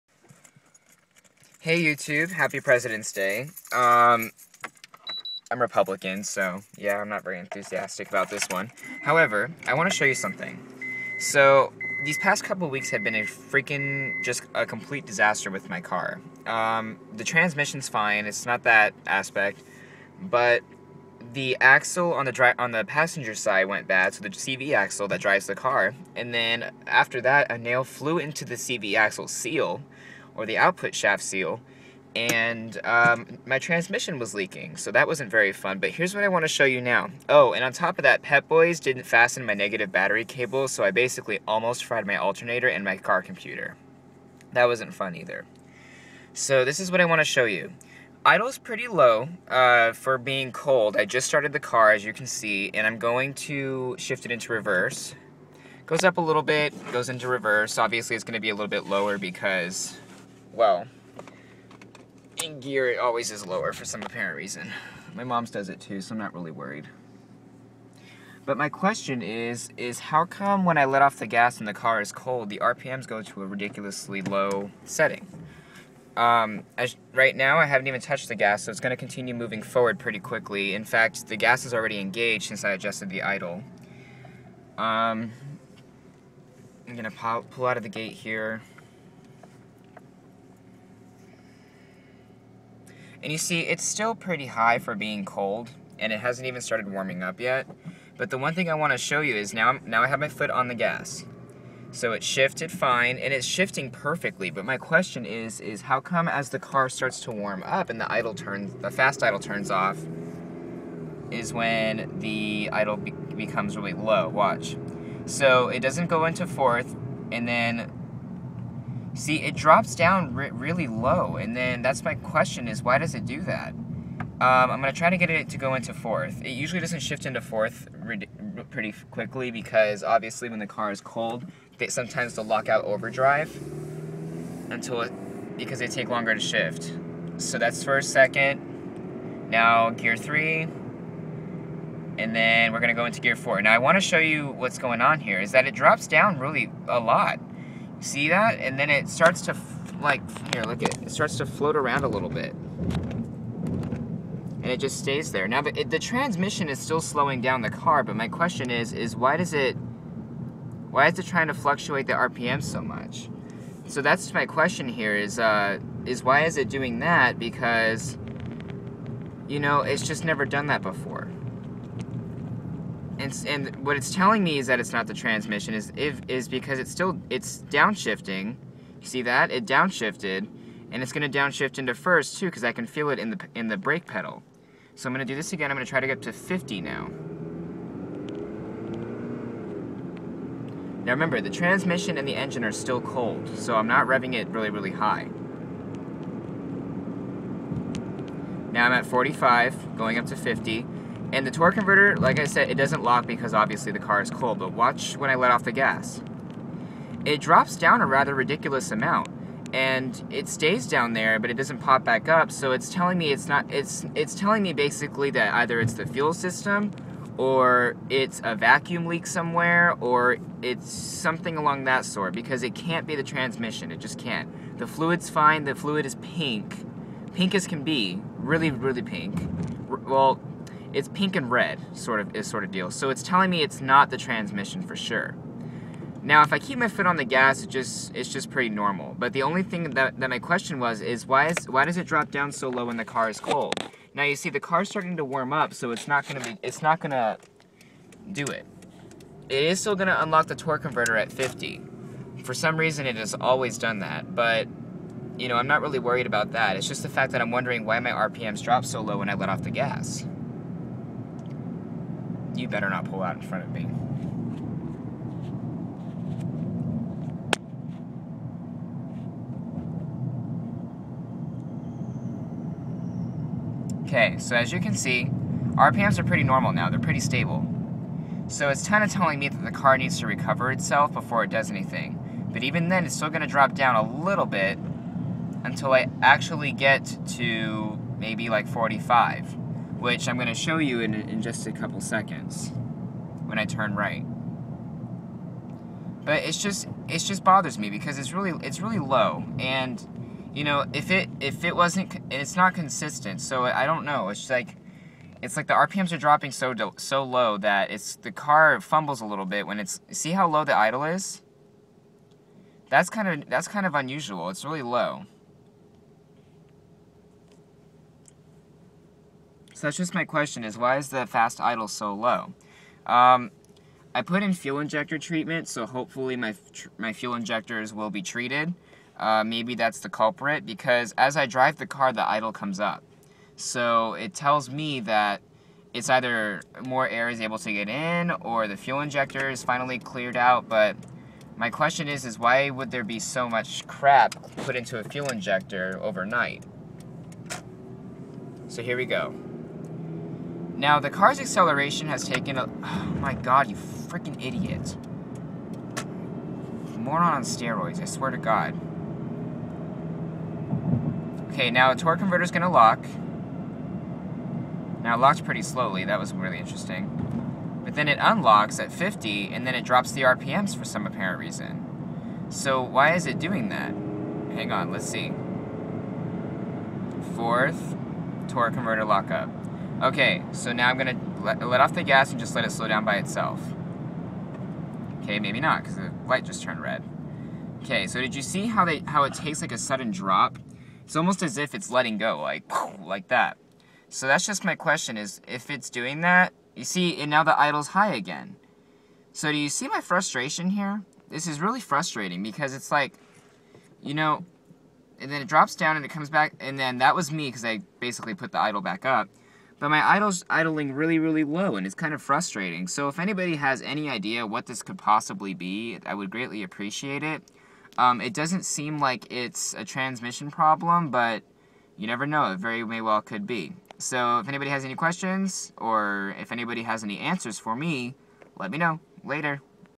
2000 Honda Accord Low RPM
Car idles low when going from cold to warm and gradually increases. Stalls a few times and letting off gas makes them drop kinda fast.